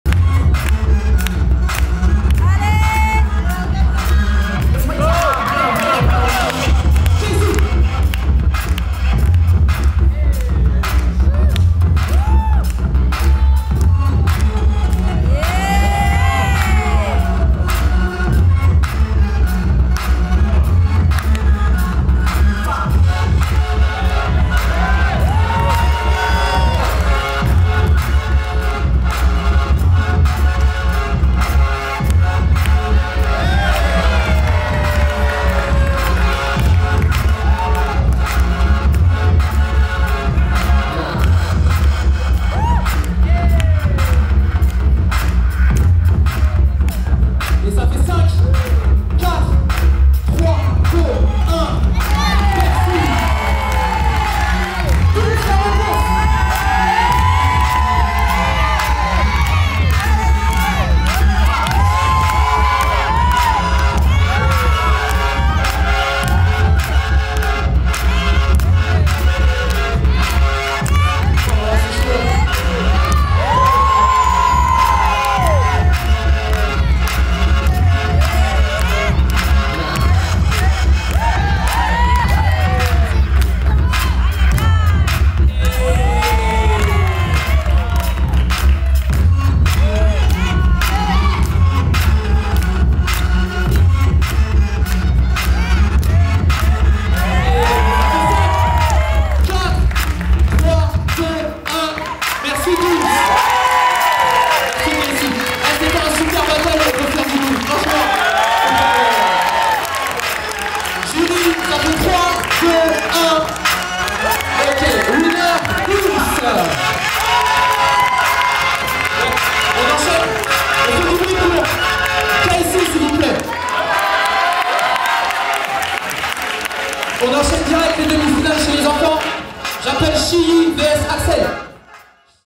KIDS (-16) - TOP 8 I DOUM'S Vs KAYSSIE I STREET ICONS BATTLE 2024 I
Mc: YANKA
Dj: Disiz GDB
Judges: FRANKWA / LAURA NALA / JOYSON

Organized by LOFT DANCE Studio (@leloftdance)
Filmed on location at Espace Gerard Philippe (Sartrouville)
Filmed & Edited by: Candyman Badness ( @candyman2965 )